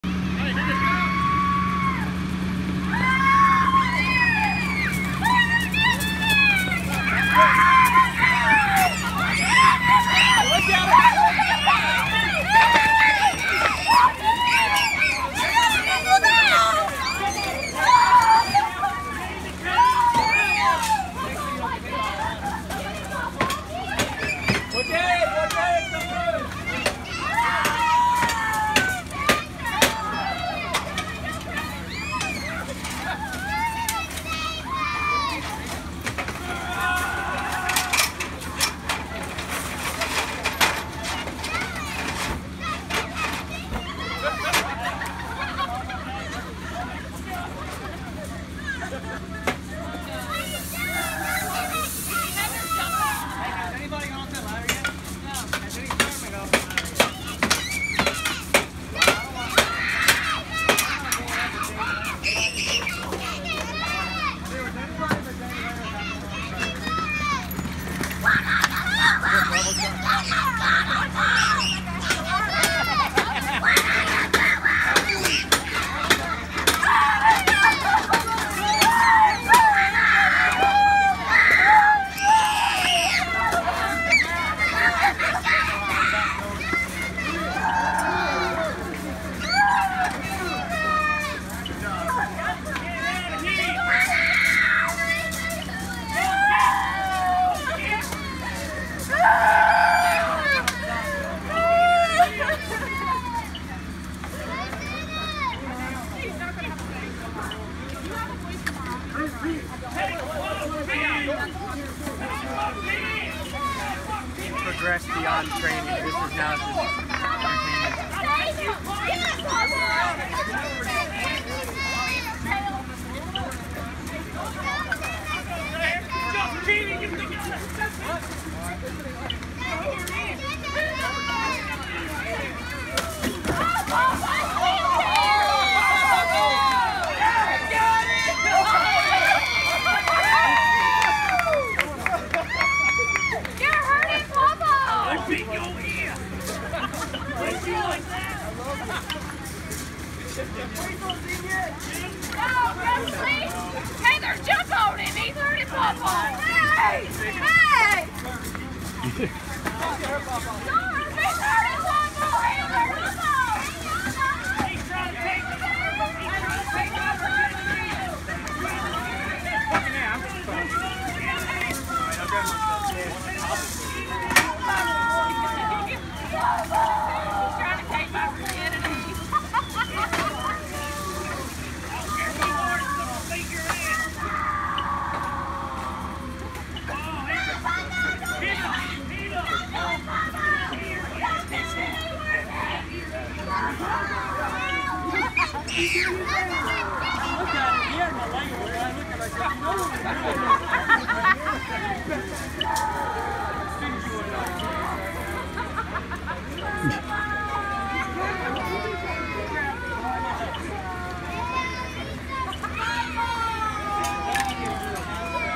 Carolina Fire Days Final Evolution
Very realistic and well put together training scenarios for the engine and truck company functions. This simulates the chaos that you don’t always count on encountering on the fire ground.